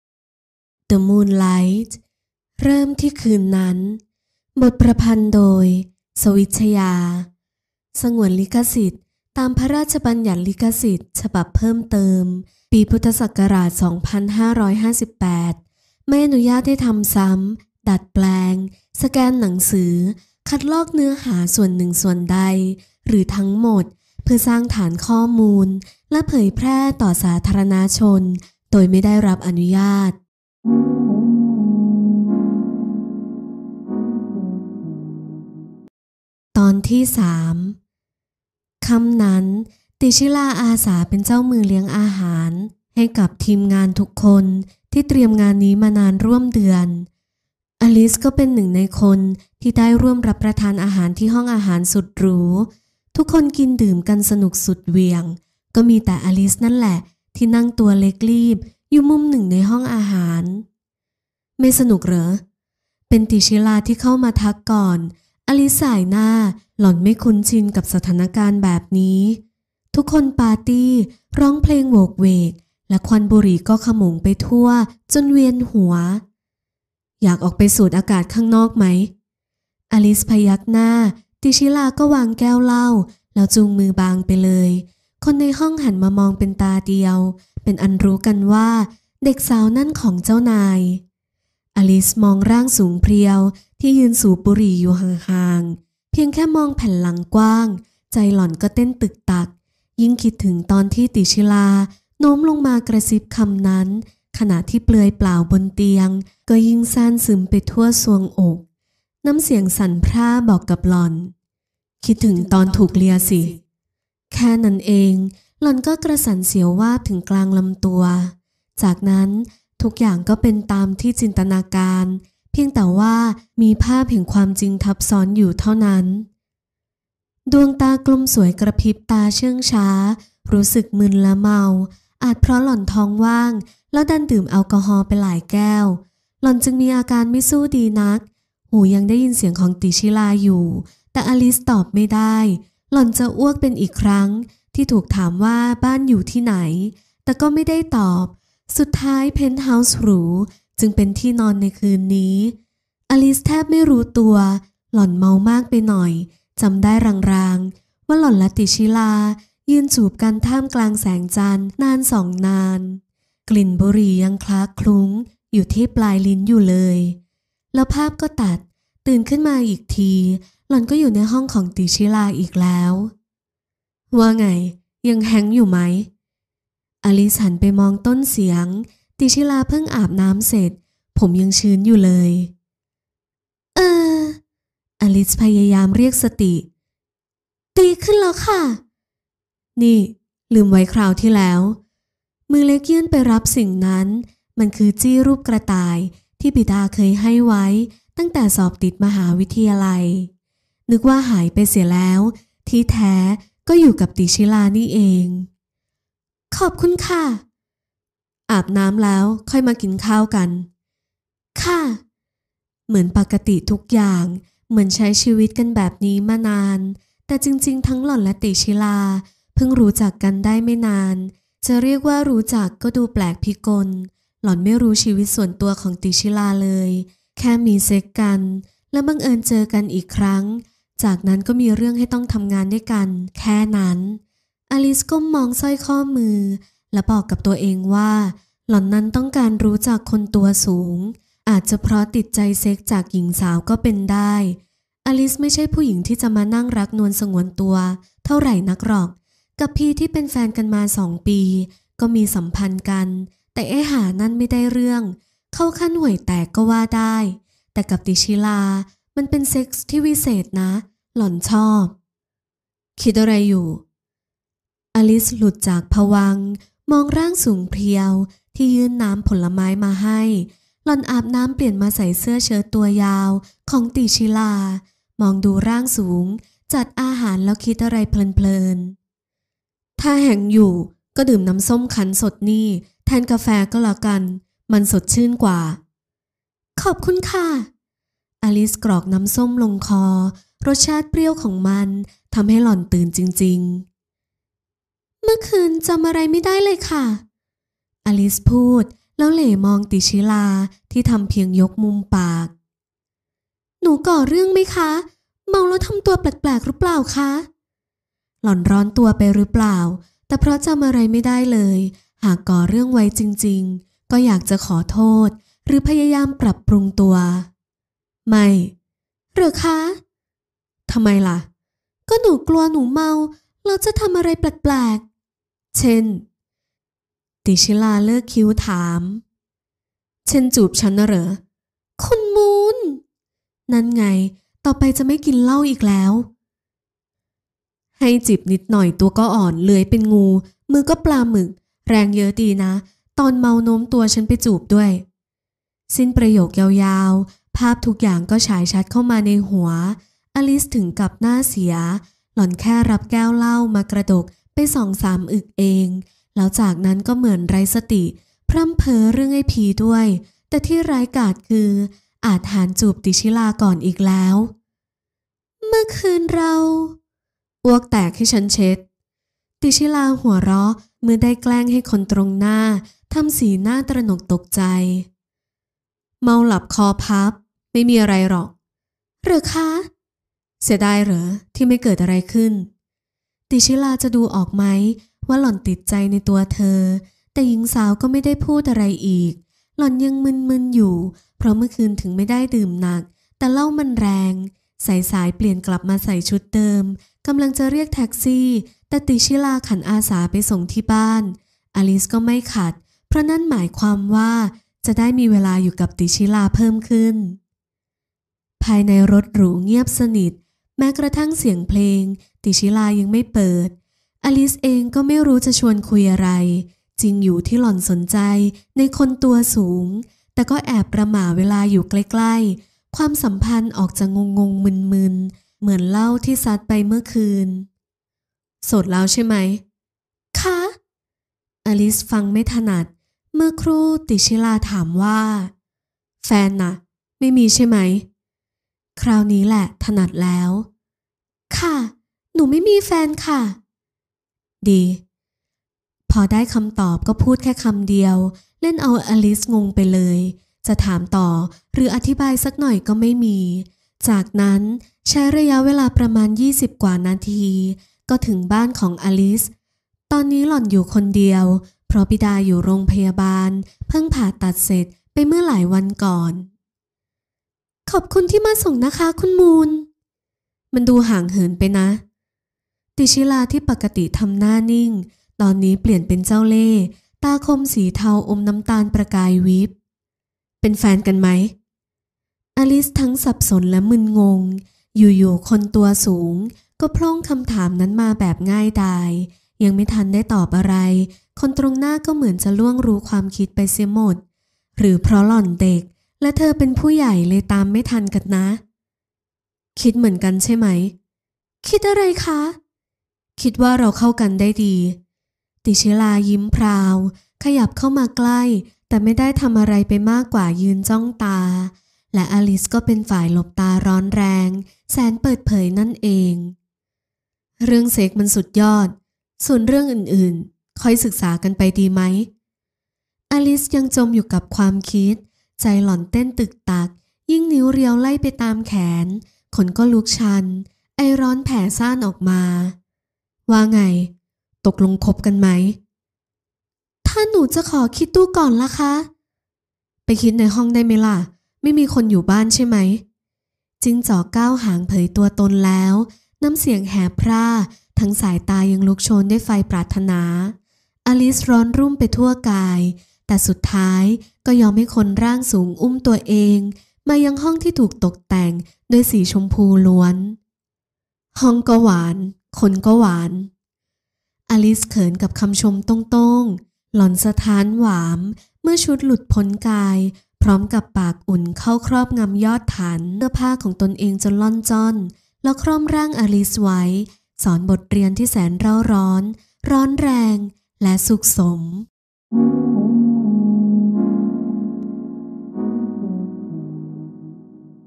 นิยายยูริ หญิงรักหญิง THE MOON LIGHT เริ่มที่คืนนั้น EP.03 | สวิชญา
พี่มูนมีนิยายเสียงแล้วน้าาา
THE MOON LIGHT เริ่มที่คืนนั้น
ดวงจันทร์ในคืนนั้นกลมโตมันทอแสงประกายระยิบ

อลิสรู้สึกมึนเมาแม้จะดื่มไปเพียงครึ่งแก้ว

ไม่กี่อึดใจที่แหงนมองแสงจันทร์หล่อนก็กระโจนจูบใส่ติชิลา

โดยไม่รู้ตัวทั้งสองมัวเมากับกลิ่นหอมหวาน

และแสงจันทร์ที่คอยสาดส่องให้กายร้อนได้ร่อนรักเข้าหากันตลอดคืน

"ไปต่อที่ห้องฉันไหม?" ติชิลากระซิบข้างหูหล่อน